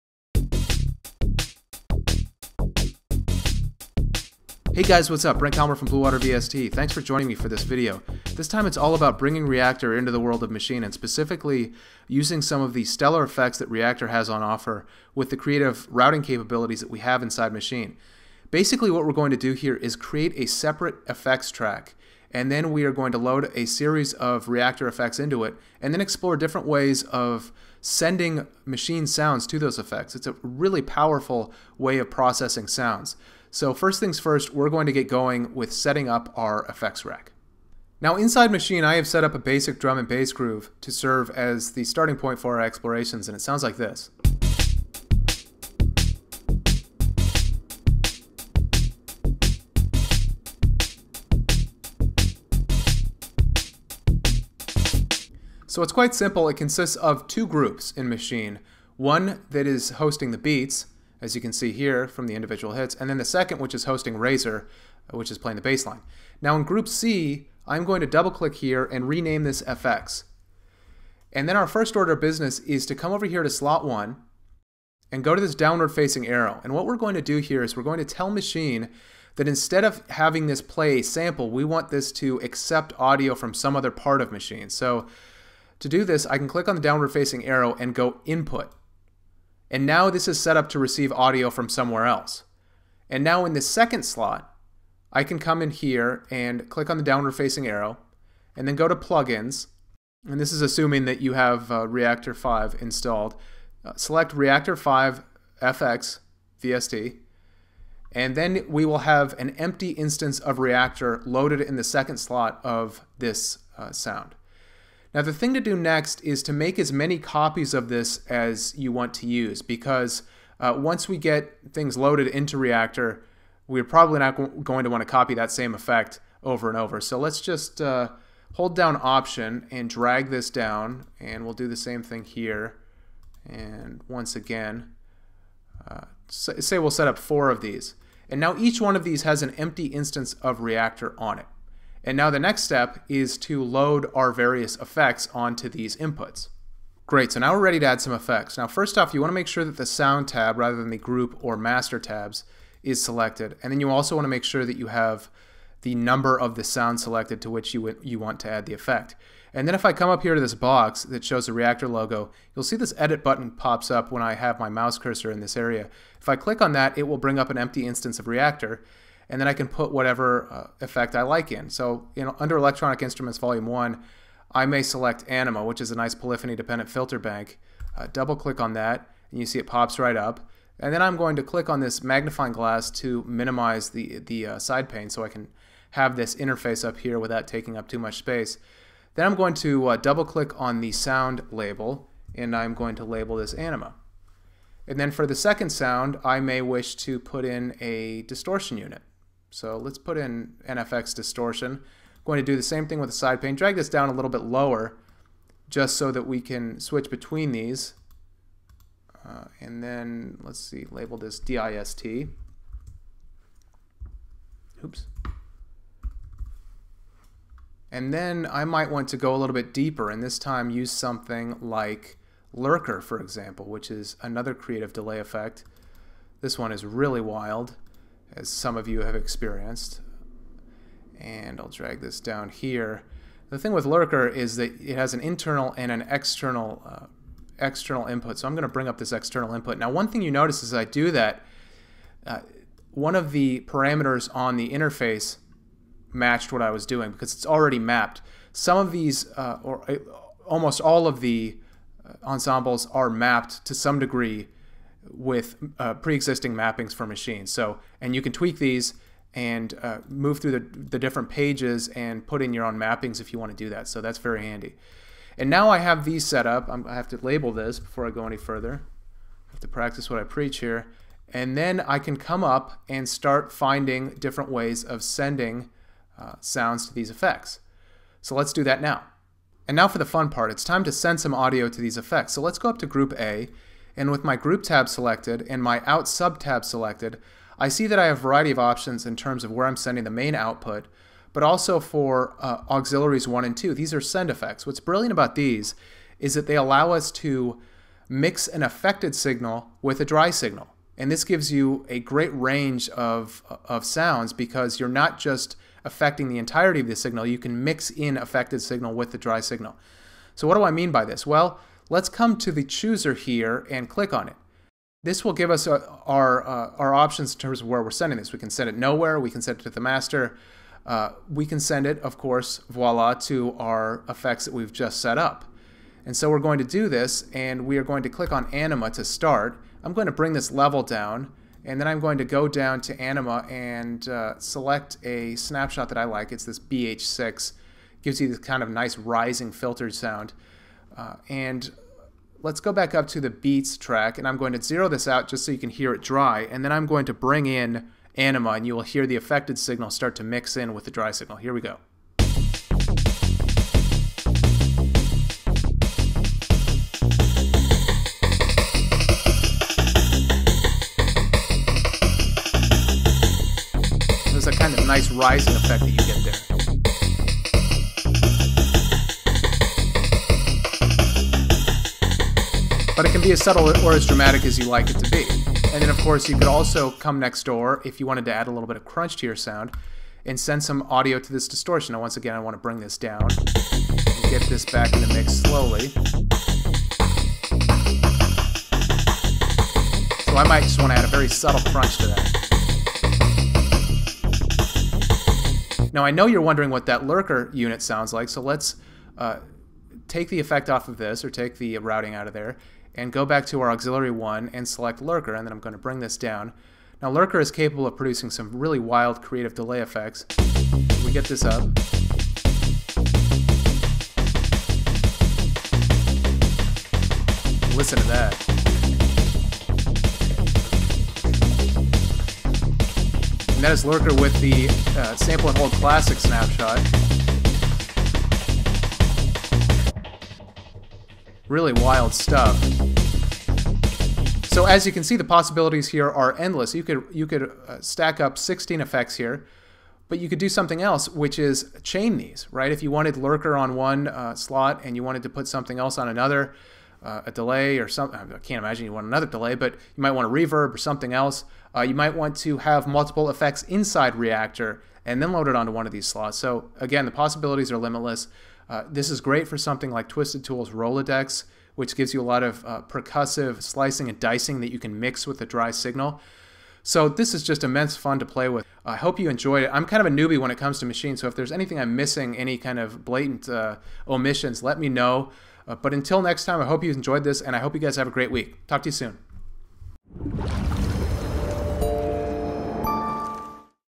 MASCHINE and REAKTOR: Stack-and-Send FX
MASCHINE and REAKTOR  form a truly devastating beatmaking team. Here's how to make the most of REAKTOR's effects inside Native Instruments' famed groove production studio.